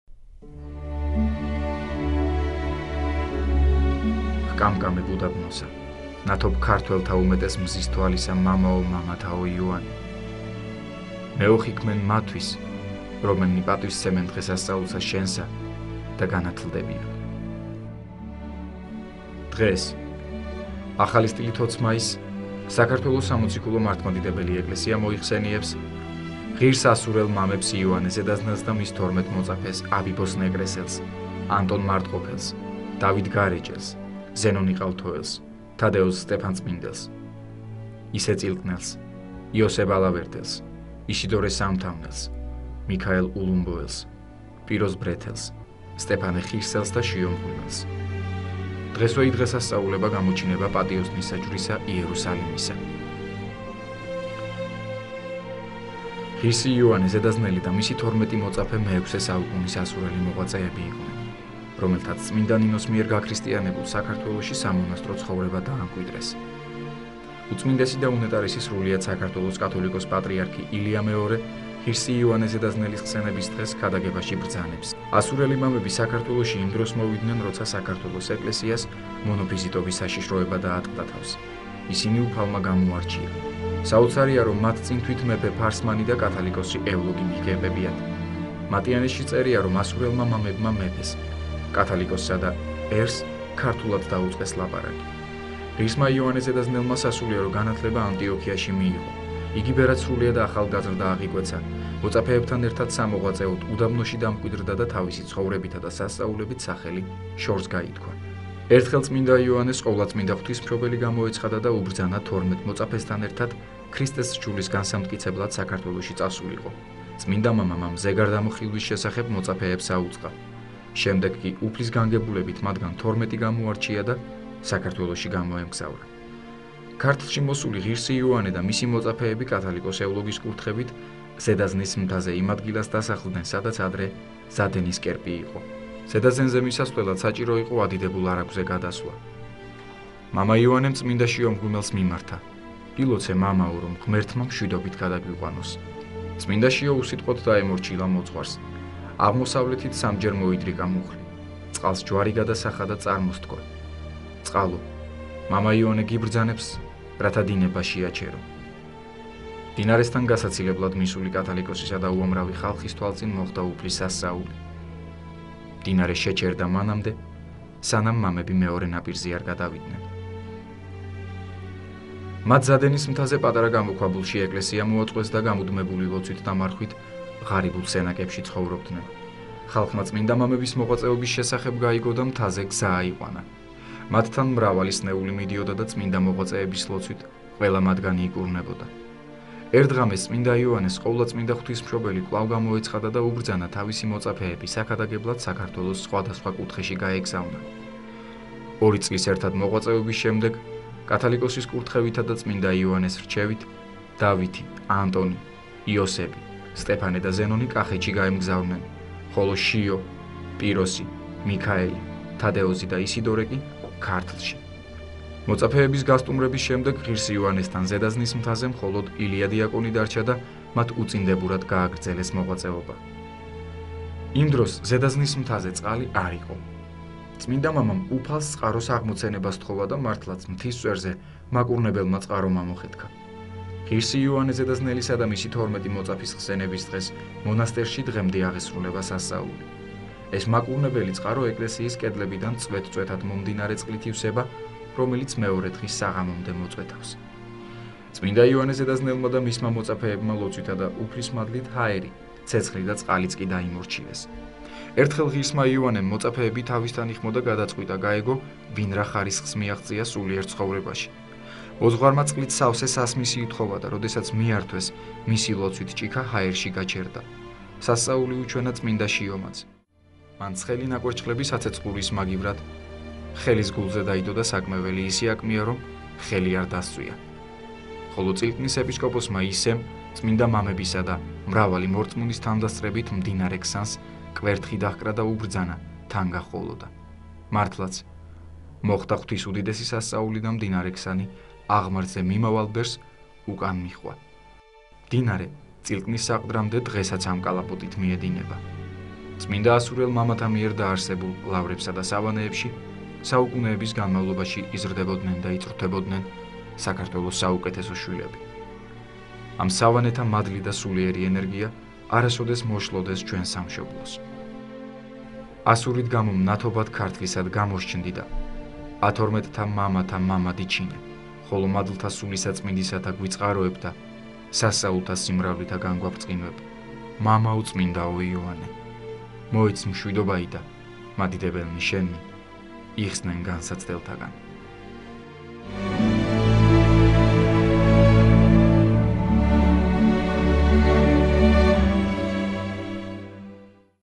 საეკლესიო კალენდარი - 20 მაისი
saeklesio kalendari - 20 maisi ; ხსენება სასწაულისა პატიოსნისა ჯვარისა, რომელი გამოცხადდა ცათა შინა იერუსალიმს (351); ღირსისა იოანე ზედაზნელისა და ათორმეტთა მოწაფეთა მისთა: აბიბოს ნეკრესელისა, ანტონ მარტყოფელისა, დავით გარეჯელისა, ზენონ იყალთოელისა, თადეოზ სტეფანწმინდელისა (თათა მამებელისა), ისე წილკნელისა, იოსებ ალავერდელისა, ისიდორე (ეზდრიოს) სამთავნელისა, მიქაელ ულუმბოელისა, პიროს ბრეთელისა, სტეფანე ხირსელისა და შიო მღვიმელისა (VI); მოწამისა აკაკი ასისთავისა (303).